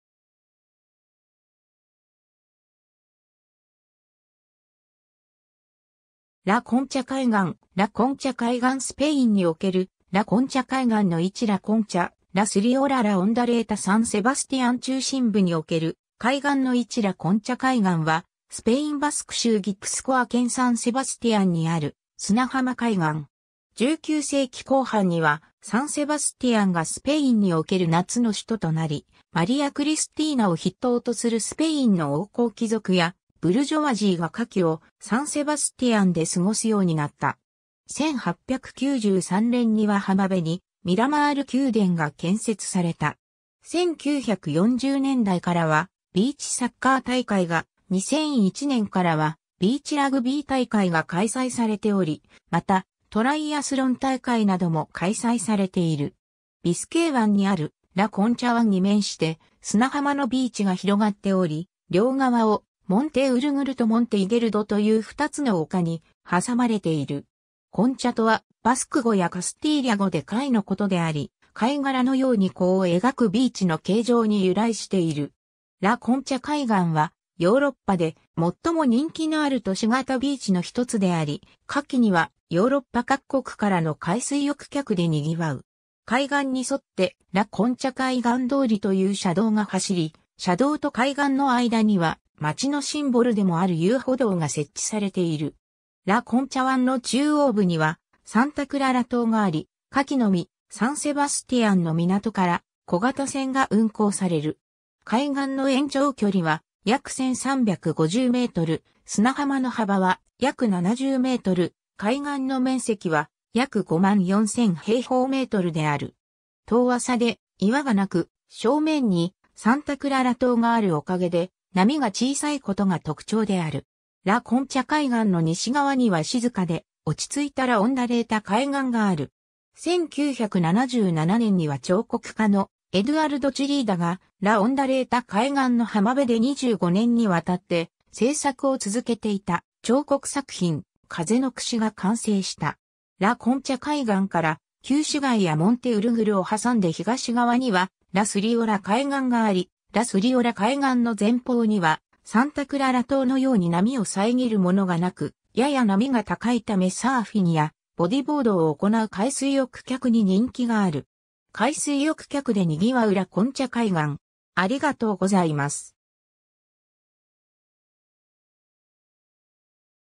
ラ・コンチャ海岸
サン・セバスティアンの地理
ラ・コンチャ海岸 ラ・コンチャ海岸 スペインにおけるラ・コンチャ海岸の位置 ラ・コンチャ ラ・スリオラ ラ・オンダレータ サン・セバスティアン中心部における海岸の位置 ラ・コンチャ海岸（バスク語: Kontxa Hondartza [ˈkontʃa onˈdarts̻a], スペイン語: Playa de La Concha [ˈplaʝa ðe la ˈkoɲtʃa]）は、スペイン・バスク州ギプスコア県サン・セバスティアンにある砂浜海岸。
 19世紀後半にはサン・セバスティアンがスペインにおける夏の首都となり、マリア・クリスティーナを筆頭とするスペインの王侯貴族やブルジョワジーが夏季をサン・セバスティアンで過ごすようになった。
1893年には浜辺にミラマール宮殿が建設された。
1940年代からはビーチサッカー大会が、2001年からはビーチラグビー大会が開催されており、またトライアスロン大会なども開催されている。
 ビスケー湾（大西洋）にあるラ・コンチャ湾に面して砂浜のビーチが広がっており、両側をモンテ・ウルグルとモンテ・イゲルドというふたつの丘に挟まれている。
コンチャ(Kontxa, Concha)とはバスク語やカスティーリャ語で貝のことであり、貝殻のように弧を描くビーチの形状に由来している。
ラ・コンチャ海岸はヨーロッパでもっとも人気のある都市型ビーチのひとつであり、夏季にはヨーロッパ各国からの海水浴客でにぎわう。
海岸に沿ってラ・コンチャ海岸通りという車道が走り、車道と海岸の間には町のシンボルでもある遊歩道が設置されている。
ラ・コンチャ湾の中央部にはサンタ・クララ島があり、夏季のみサン・セバスティアンの港から小型船が運航される。
海岸の延長距離は約1,350m、砂浜の幅は約70m、海岸の面積は約54,000m2である。
遠浅で岩がなく、正面にサンタ・クララ島があるおかげで波が小さいことが特徴である。
 ラ・コンチャ海岸の西側には静かで落ち着いたラ・オンダレータ海岸がある。
1977年には彫刻家のエドゥアルド・チリーダがラ・オンダレータ海岸の浜辺で25年に渡って製作を続けていた彫刻作品『風の櫛』が完成した。
 ラ・コンチャ海岸から旧市街やモンテ・ウルグルを挟んで東側にはラ・スリオラ海岸があり、ラ・スリオラ海岸の前方にはサンタ・クララ島のように波を遮るものがなく、やや波が高いためサーフィンやボディボードを行う海水浴客に人気がある。
 海水浴客でにぎわうラ・コンチャ海岸